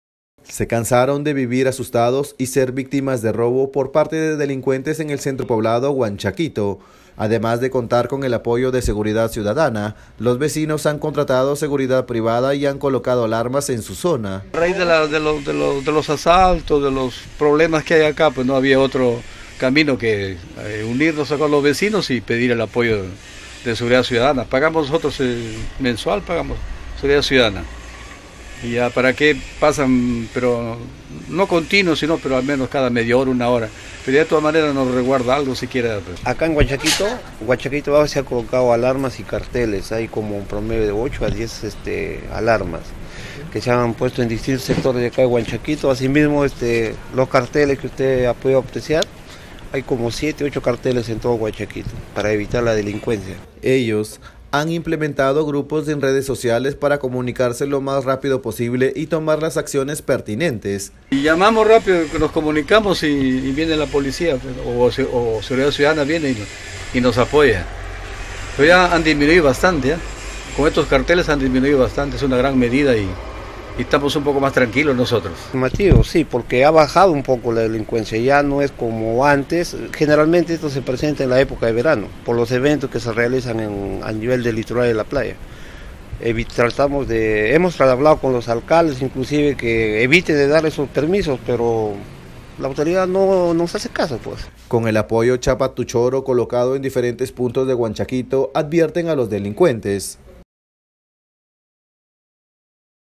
NOTA VECINOS DE HUANCHAQUITO UNIDOS CONTRA LA DELINCUENCIA
Moradores del centro poblado de Huanchaquito colocaron carteles con el mensaje “Chapa tu choro: Vecinos unidos contra la delincuencia”, debido a los continuos robos y asaltos que se registran en la zona. Además, anunciaron un trabajo conjunto con Seguridad Ciudadana.